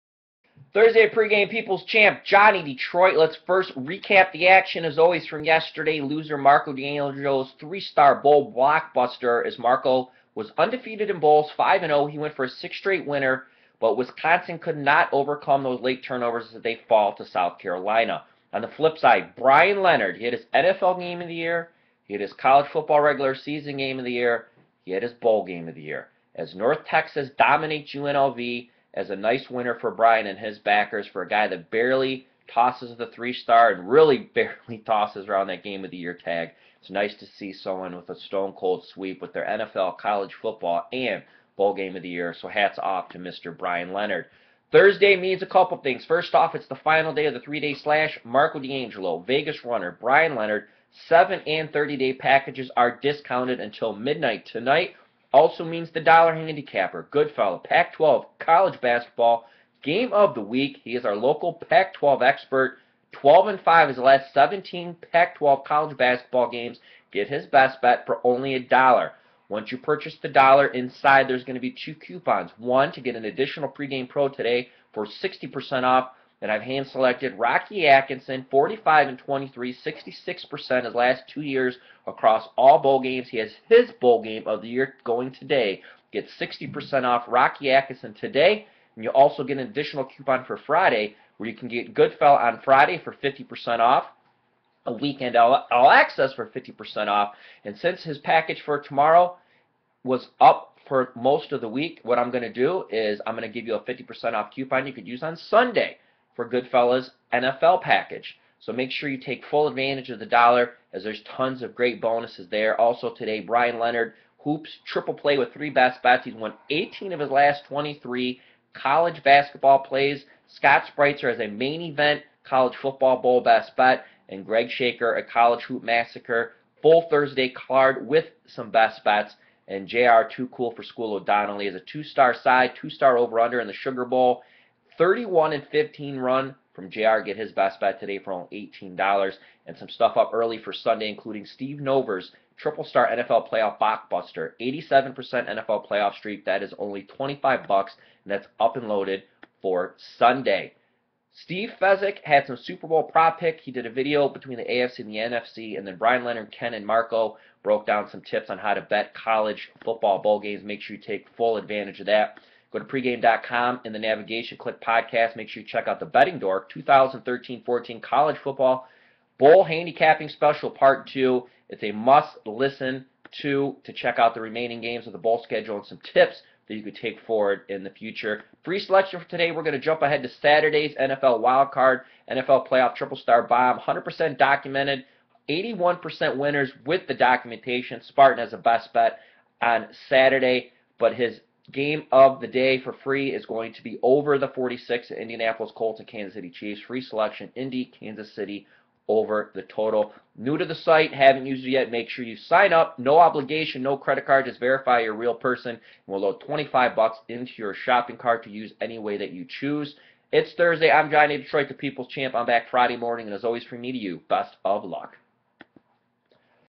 Thursday at Pregame.com (Free Sports Betting Picks- 01/02/2013)
Join host Johnny Detroit to hear about the biggest sports betting picks and best bets from the famous handicappers available for sale at Pregame.com for NFL, College Football and MLB action on Thursday, December 2, 2013.  You will also receive a Free Play courtesy of one of our Handicappers.(College Basketball, NBA,NFL,College Football, NHL, MLB, Baseball).